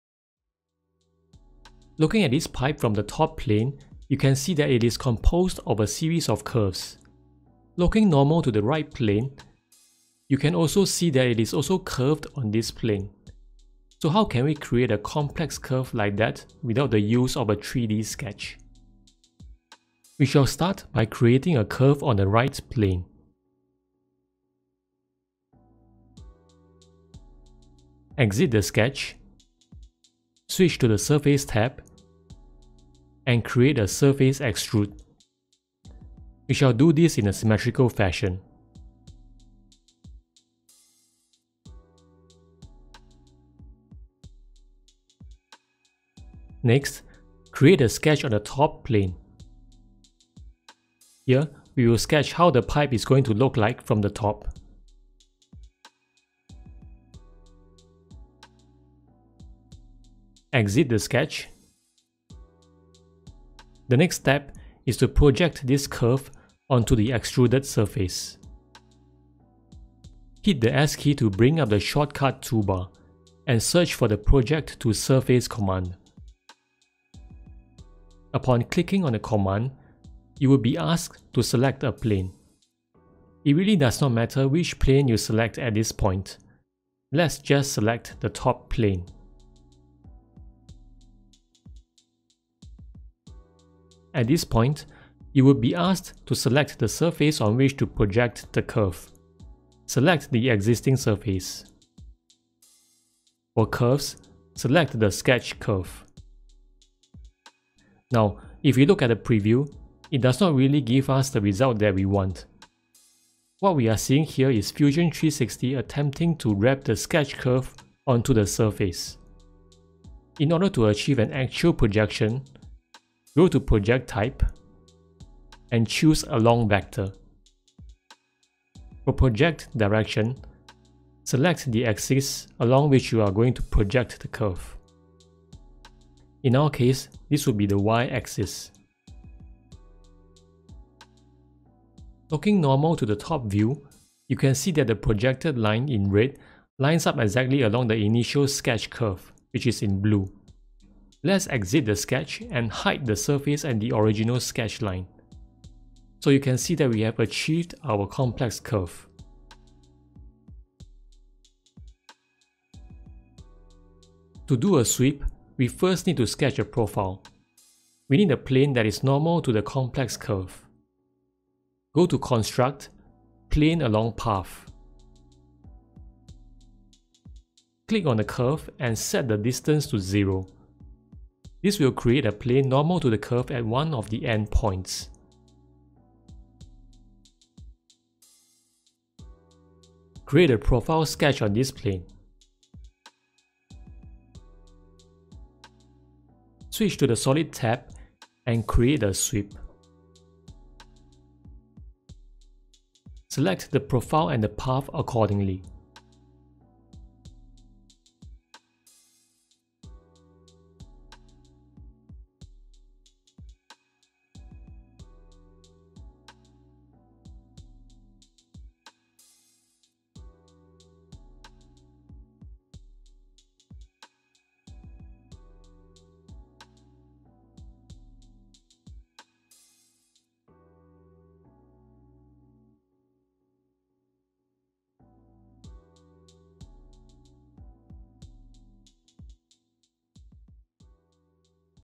Fusion 360 - Project To Surface (Complex Curves)
Creating a complex curve using project to surface.

15 July 2020: The method shown in this video is not the fastest way. We can actually use the intersect command to project 2 curves onto each other to get the complex curve. You can refer to the following video on how to use the intersect command.

Also, we can use the pipe command to do the sweep without the need for sketching a profile. At the time that this video was made, I was unaware of these 2 features. Apologies for this.